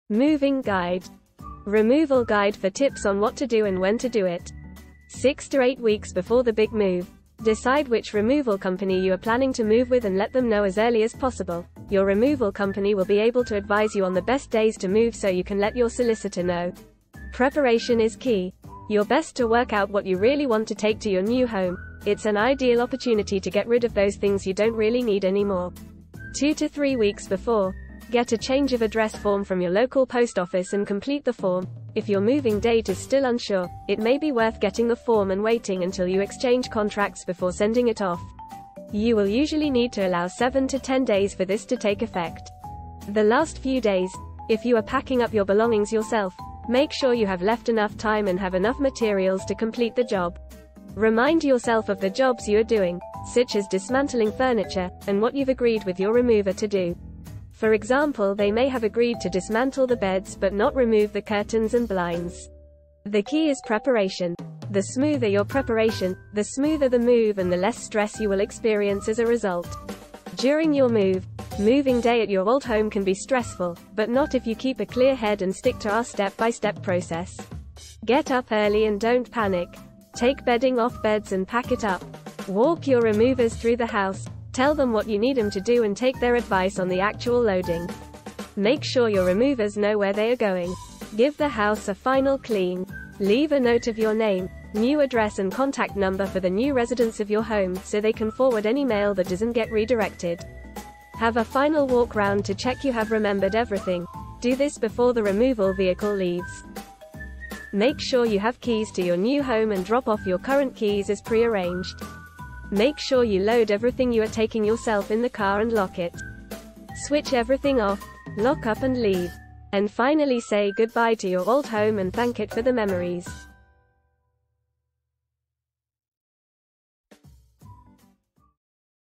Moving Guide | Top Tips on Moving House | AJ Stephenson Removals
Moving Guide for top tips on what to do and when to do it! Moving house is stressful, but this moving guide will help your move run more smoothly. Let's take the stress out of moving and move with AJ Stephenson Removals of Billericay. Billericay House Removals.